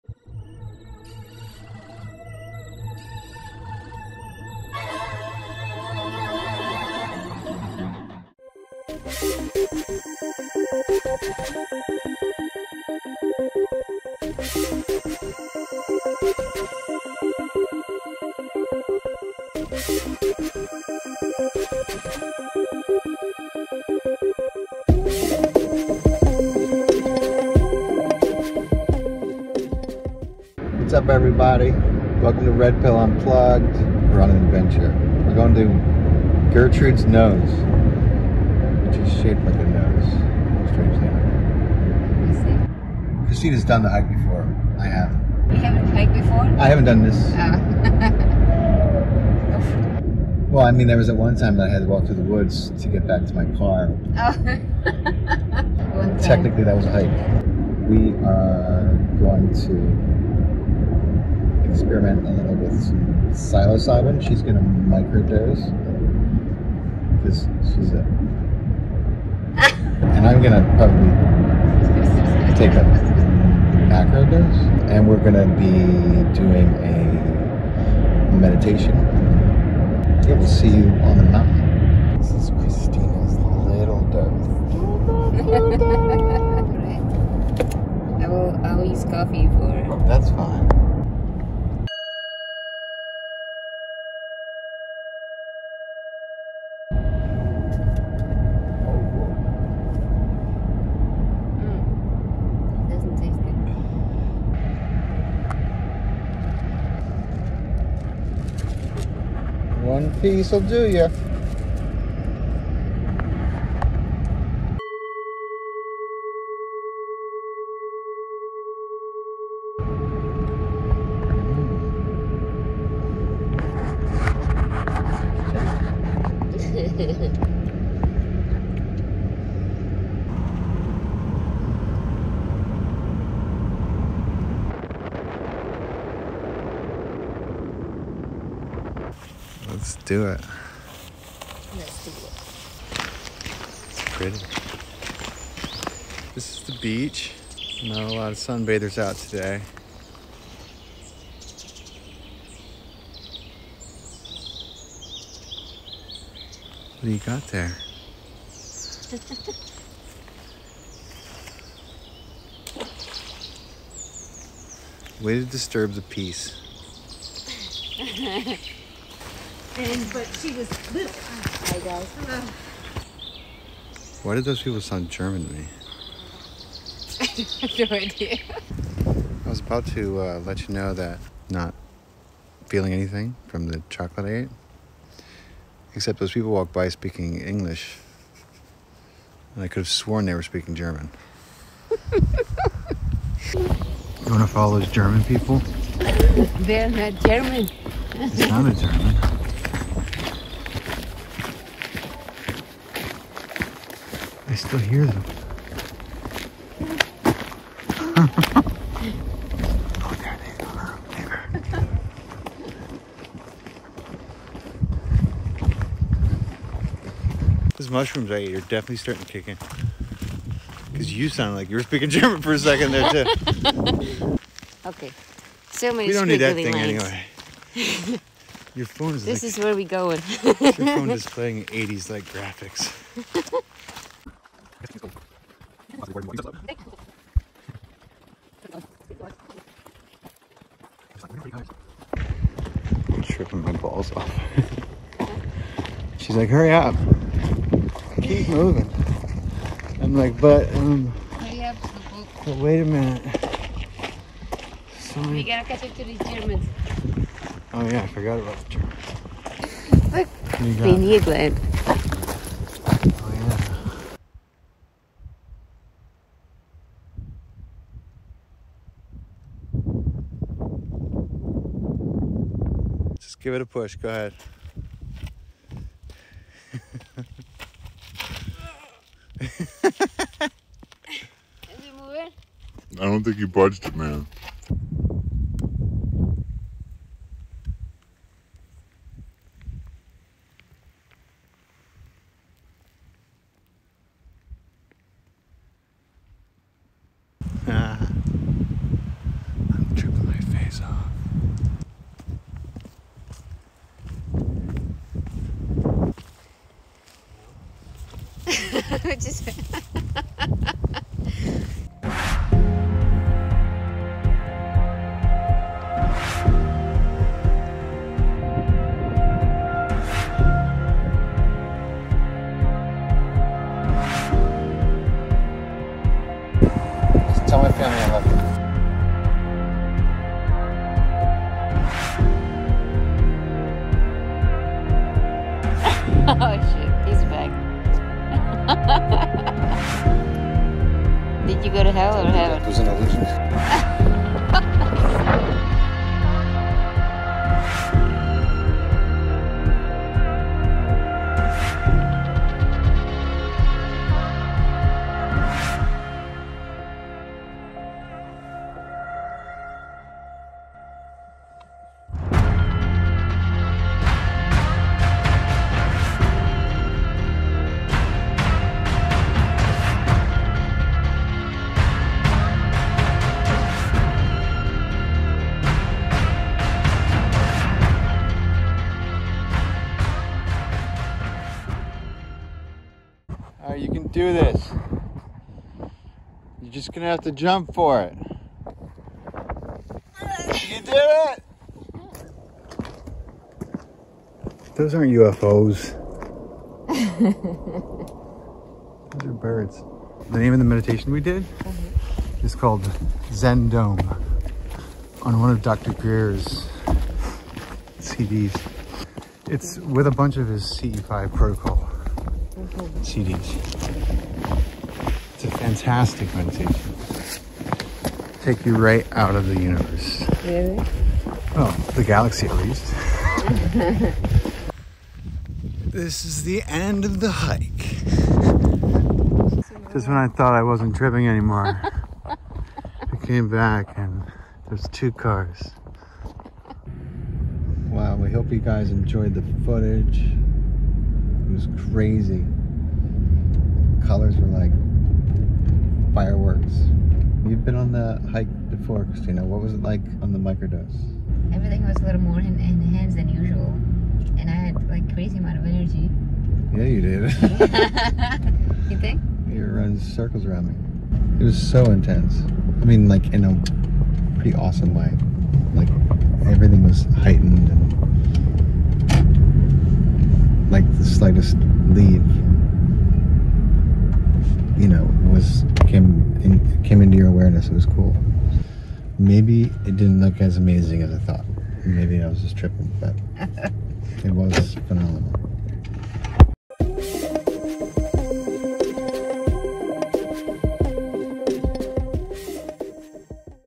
Faust and Kristyna Microdose Psilocybin | Red Pill Unplugged | VLOG 002
Faust and Kristyna take micro and macro doses of Psilocybin🍄 on an epic 8 mile Trip in upstate New York all for an "out there" UFO meditation.